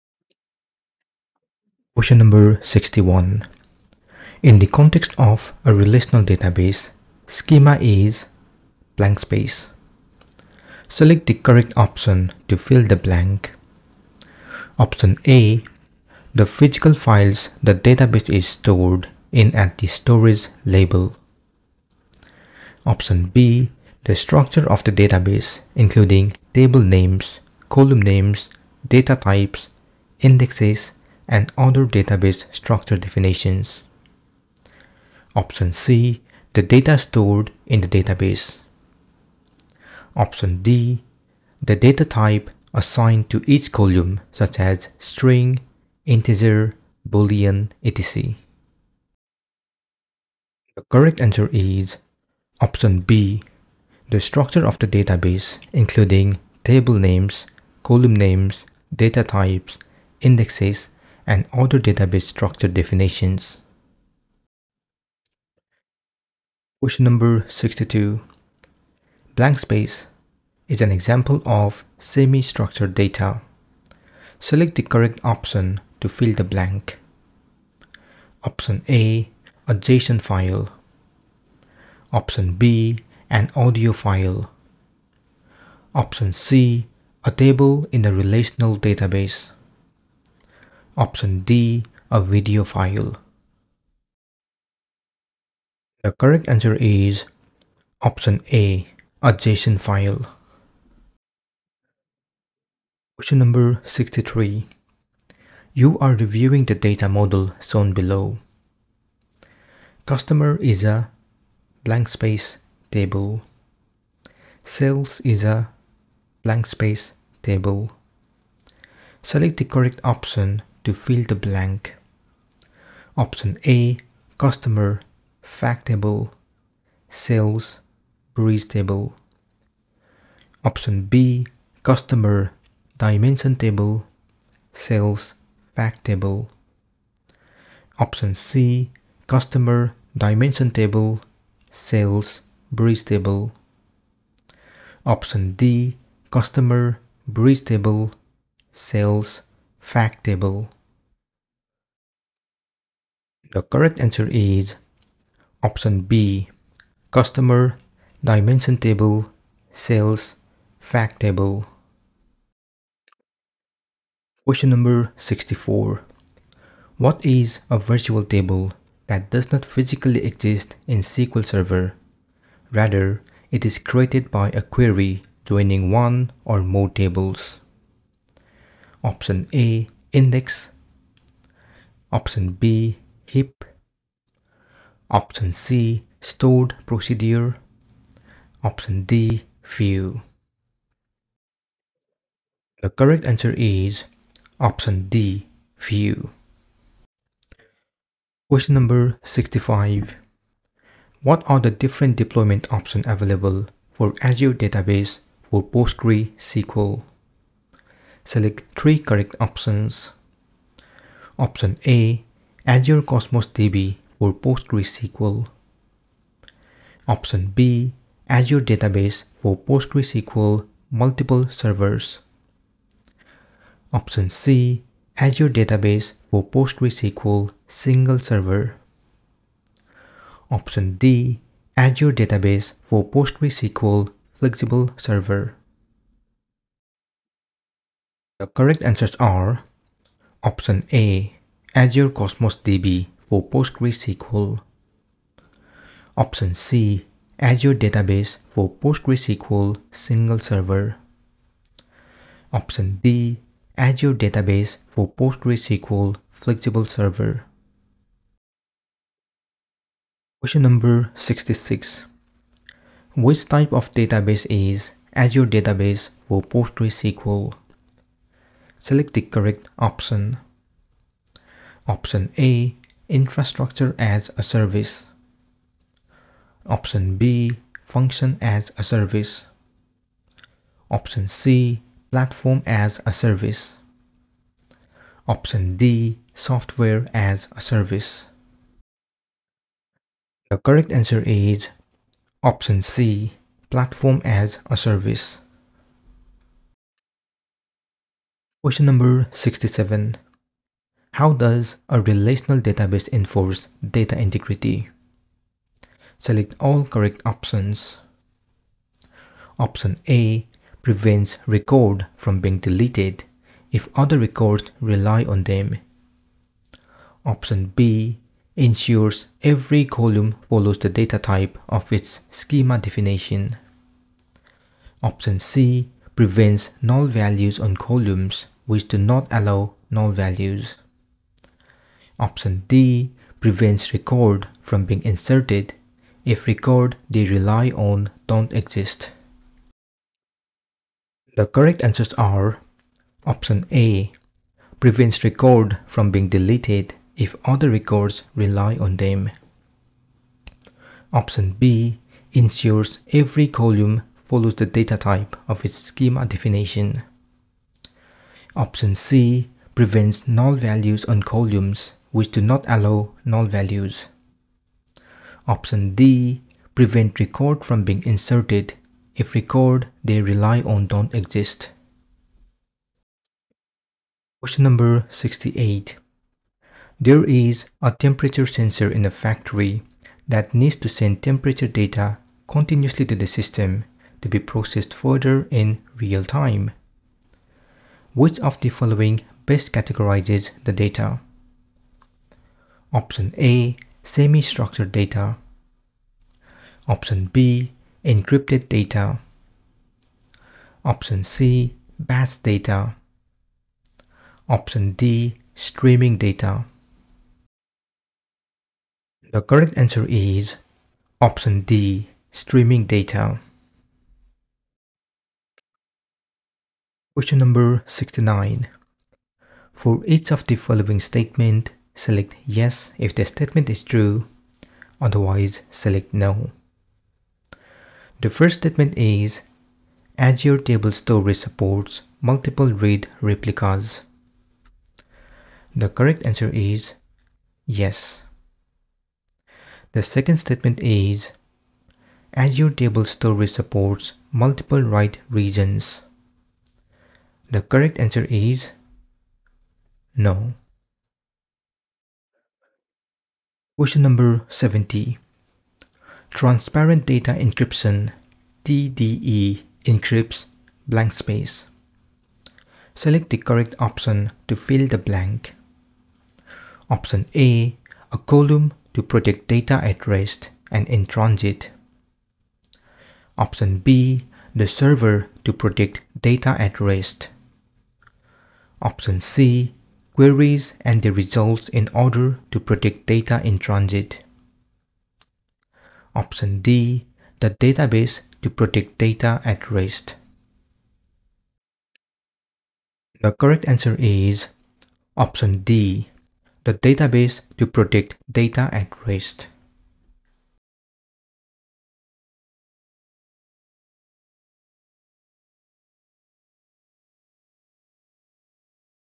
DP 900 Exam Q&A #7 - Azure Data Fundamentals
Introduction:
Prepare for the DP-900 exam with 210 expertly curated questions and answers! This comprehensive guide covers all key topics to help you ace the Microsoft Azure Data Fundamentals certification. Perfect for beginners and professionals looking to validate their data skills. Start your journey to becoming Azure certified today!